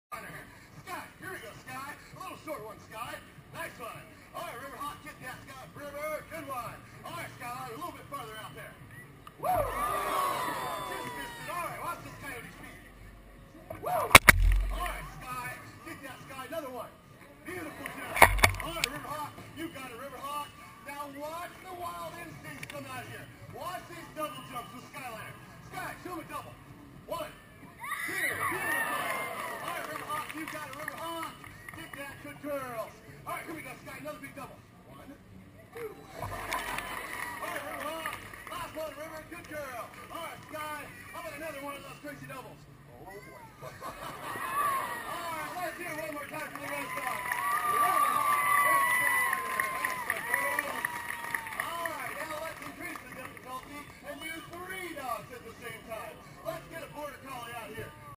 Filming with a GoPro HD Hero2 at ground level for the K9 Kings Flying Dog Show
Sweet ground level footage of the K9 Kings Flying Dog Show featuring American Indian Dogs "Riverhawk" and "Skyliner".  Filmed 100% with a GoPro HD Hero2.